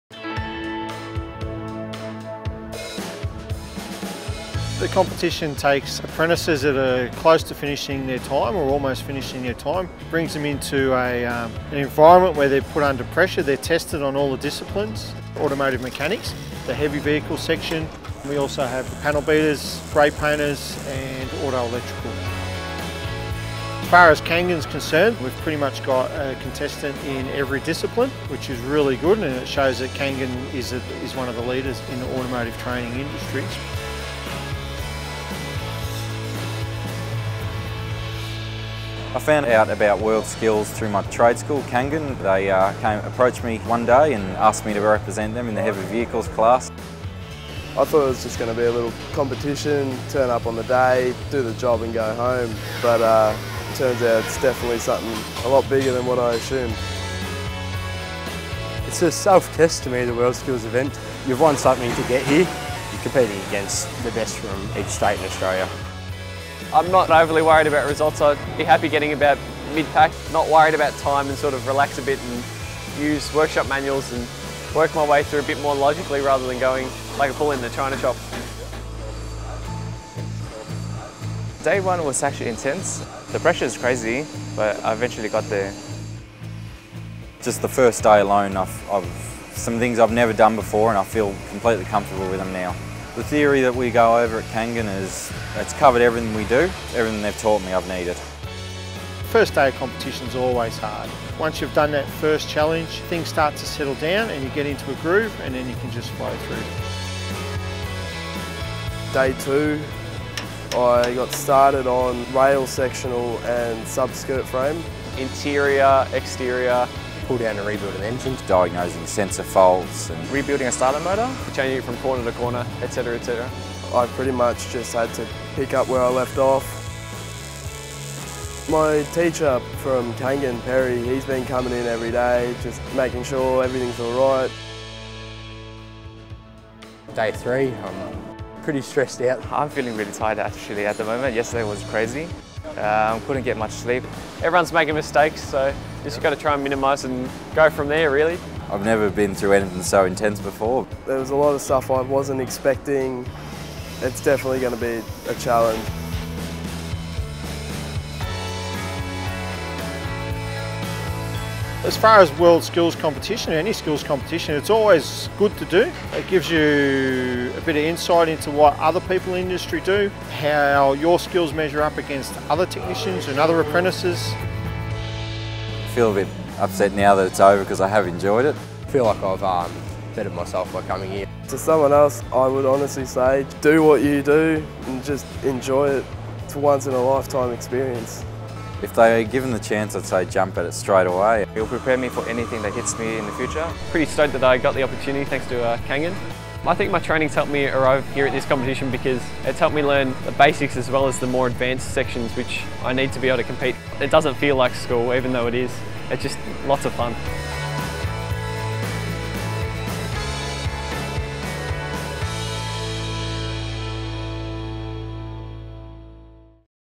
Kangan @ World Skills - Automotive
Check out our Automotive students competing at the World Skills 2016 National Finals.